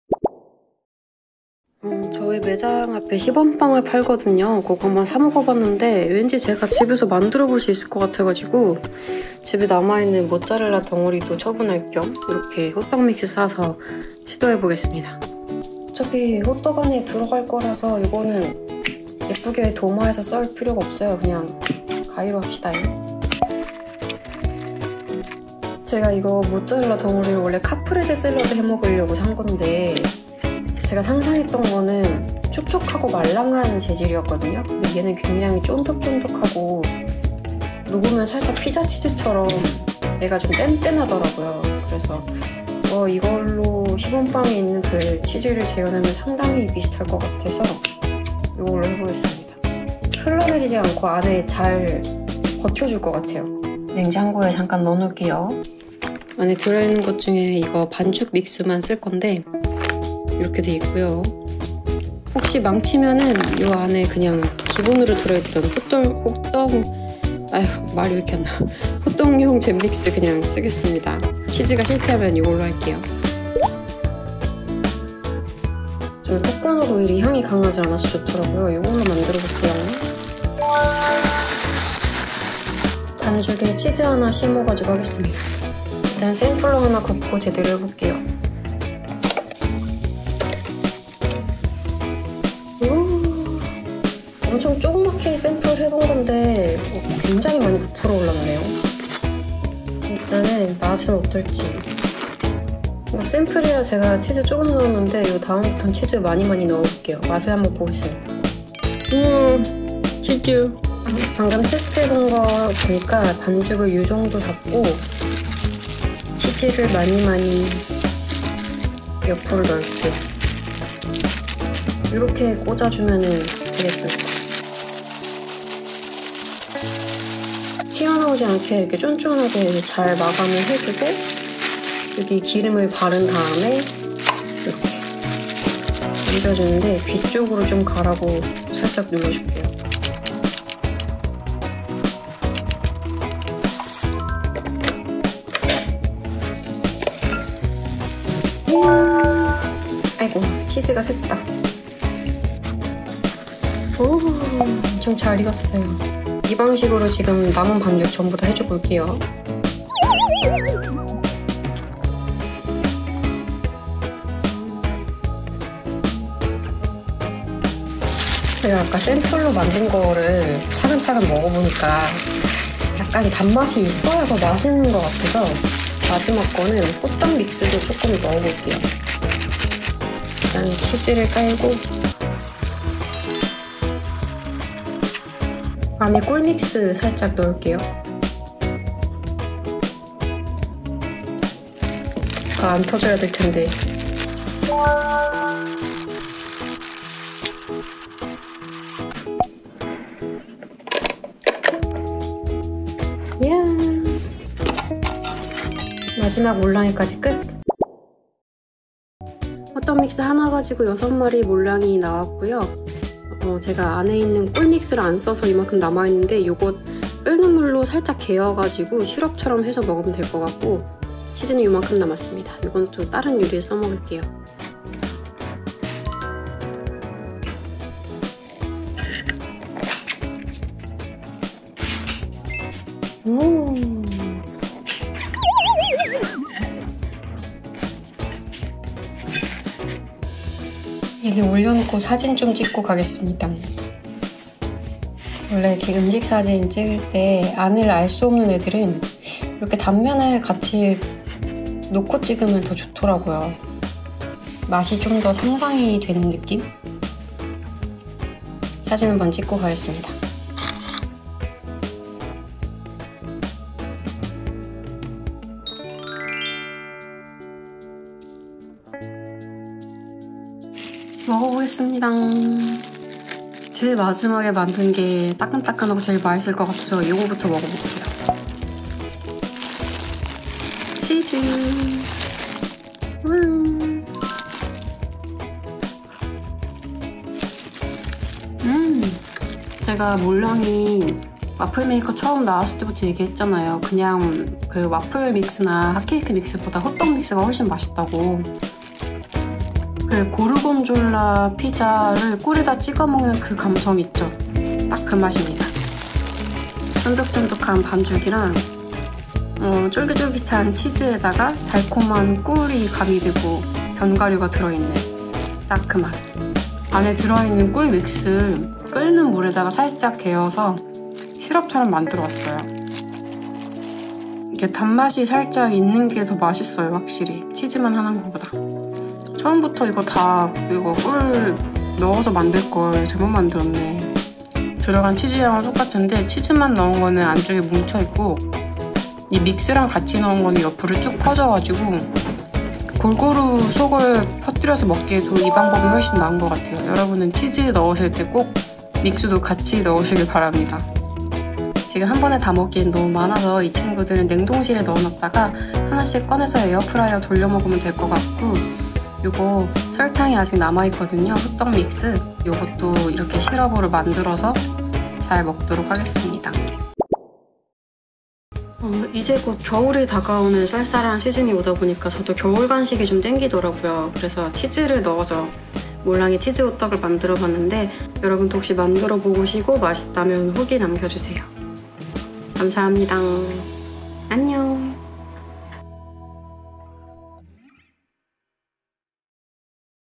몰랑이 치즈호떡 만들어먹기
날씨가 쌀쌀해지면 돌아오는 몰랑이 호떡 만들기ㅎㅎ
우리 매장 근처 십원빵 먹다가 치즈 들어간 달달빵 조합이 맛있길래 집에서 해봤어요 :)